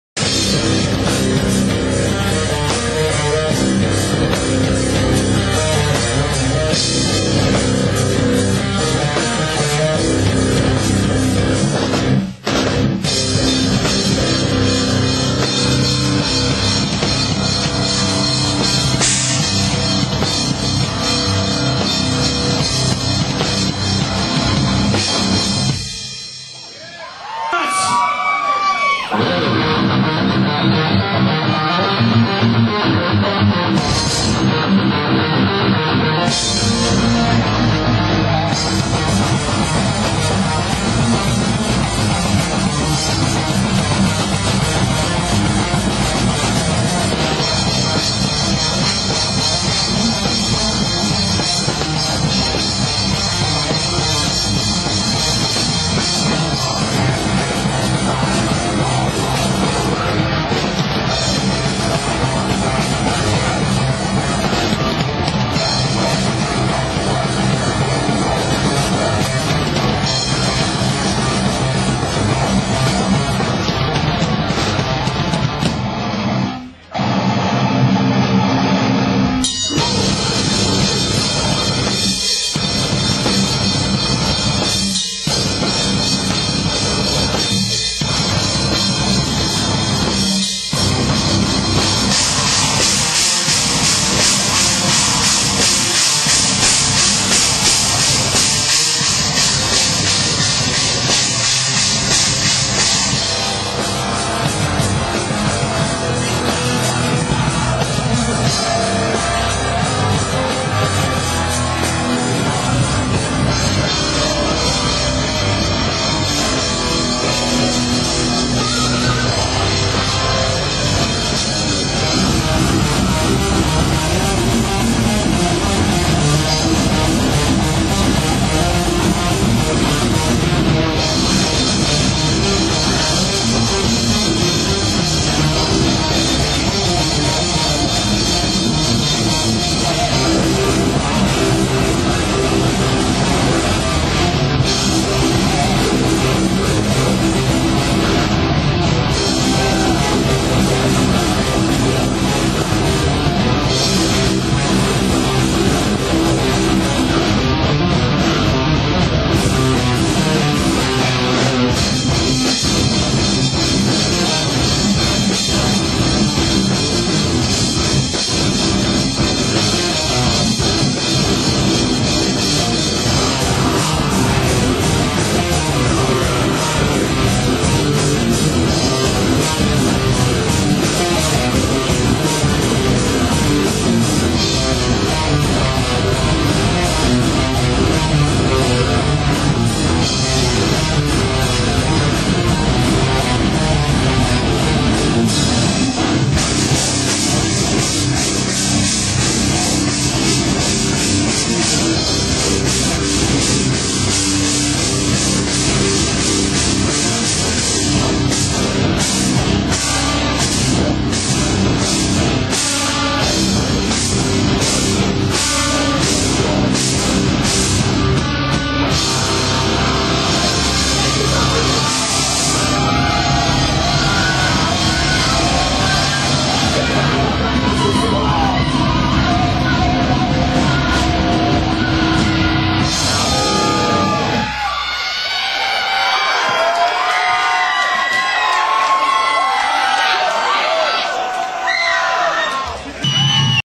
SlamHandle
two more vids of this band will be put up, they are the second first place winners of the battle of the bands at Jesters.